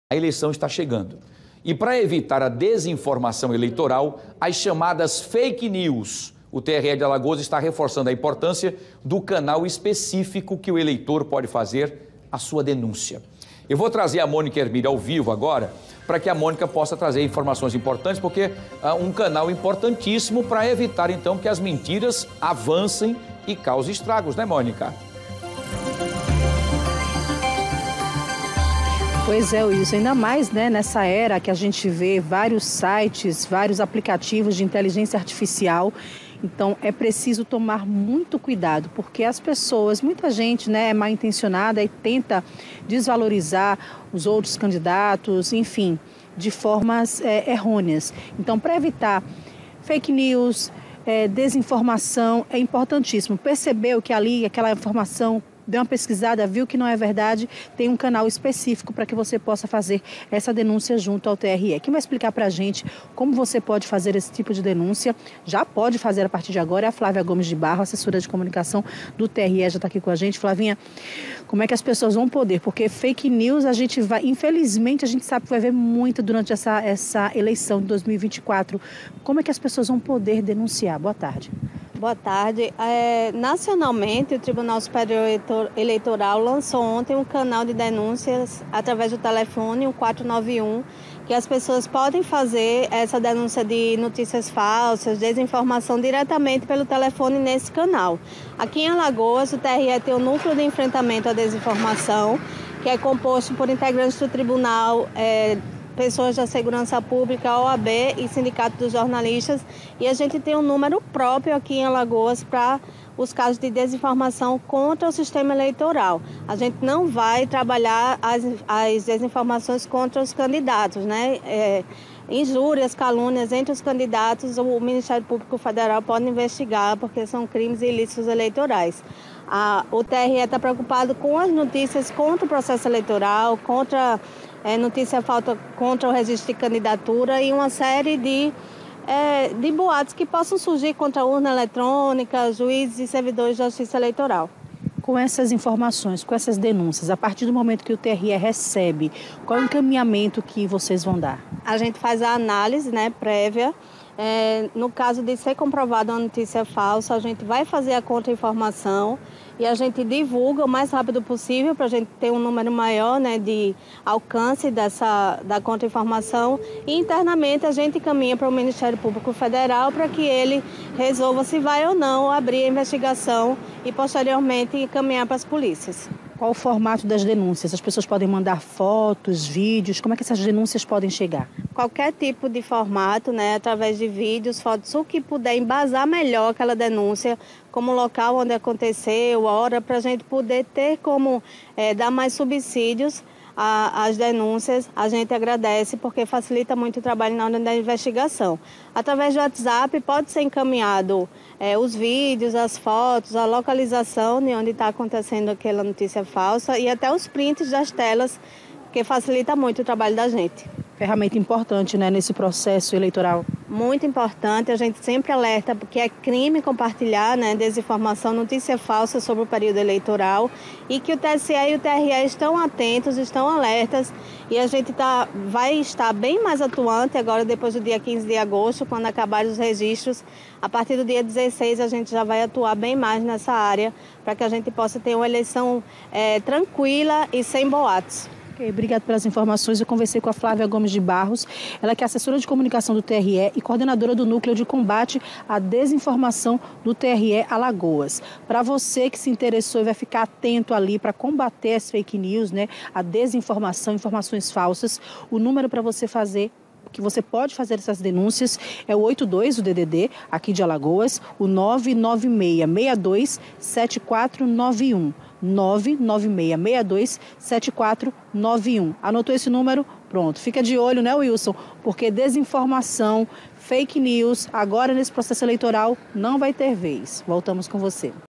TRE/AL reforça importância de evitar a desinformação eleitoral, as chamadas Fake News
Assessora de comunicação TRE, Flávia Gomes de Barros, explica como fazer a denúncia.